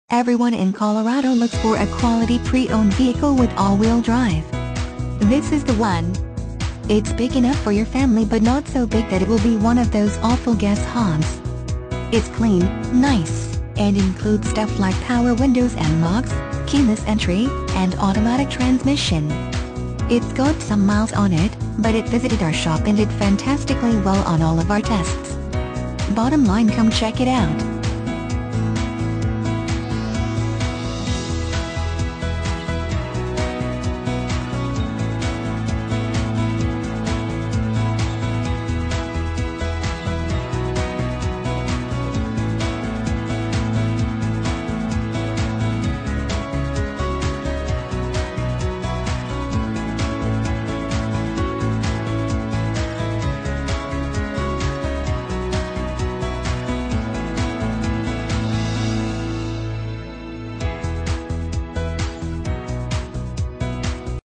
2006 NISSAN MURANO Greeley, CO 171681A
2006 NISSAN MURANO Greeley, CO
Stock

Peak Kia of Greeley Used
3803 West 10th St

EVERYONE in Colorado looks for a quality pre-owned vehicle with All Wheel Drive.  This is the one!  Its big enough for your family but not so big that it will be one of those awful gas hogs.  Its clean, nice, and includes stuff like power windows and locks, keyless entry, and automatic transmission!  Its got some miles on it, but it visited our shop and did fantastically well on all of our tests.  Bottom linecome check it out!